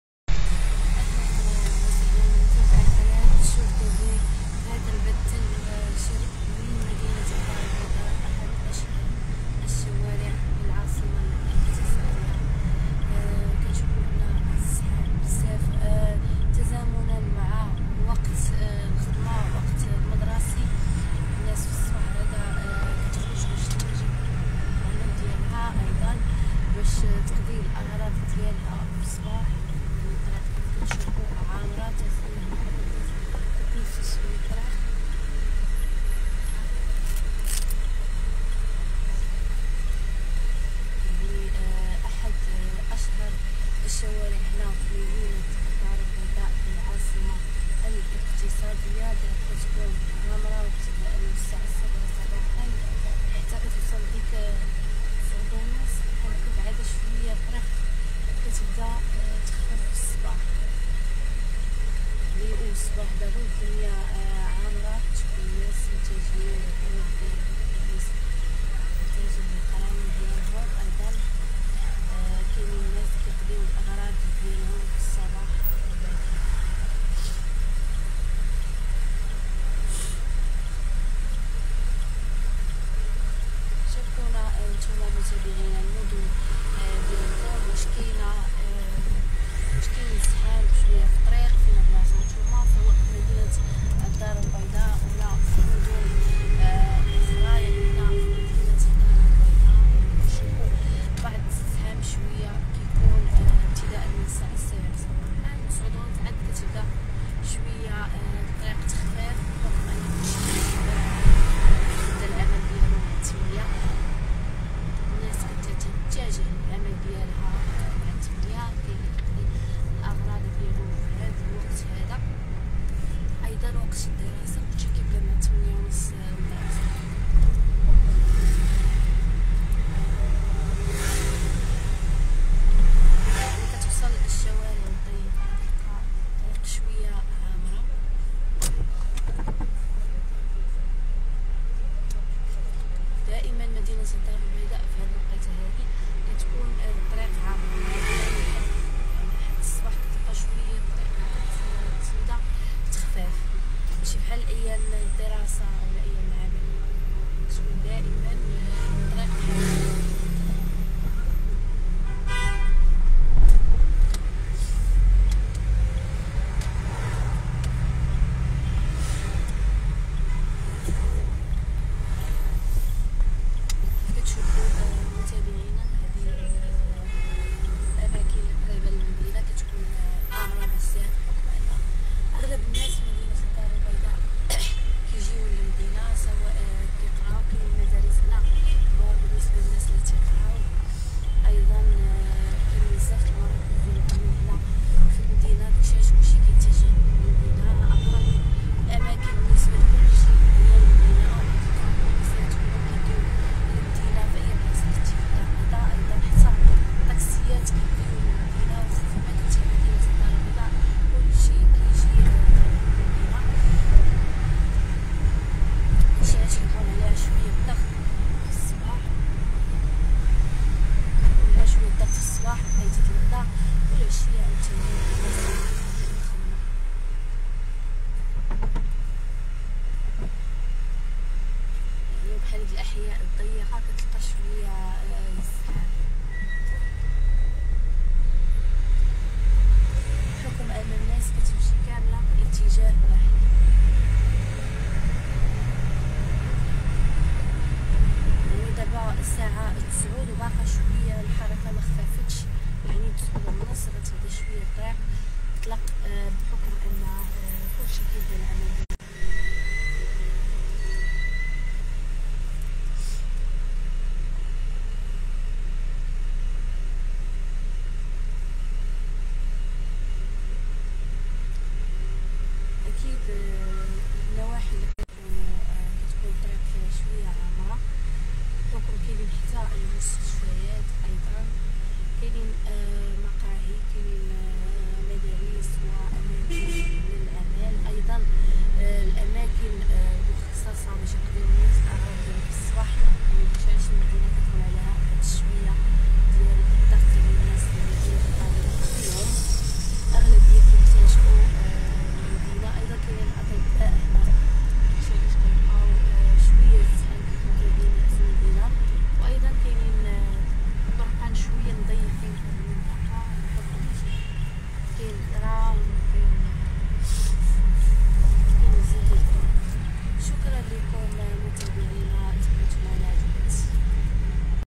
مباشرة من الدارالبيضاء: أشهر شوارع العاصمة الإقتصادية عامرة بزاف تزامننا مع وقت الدروة
مباشرة من الدارالبيضاء: أشهر شوارع العاصمة الإقتصادية عامرة بزاف تزامننا مع وقت الدروة...شوفو شنو واقع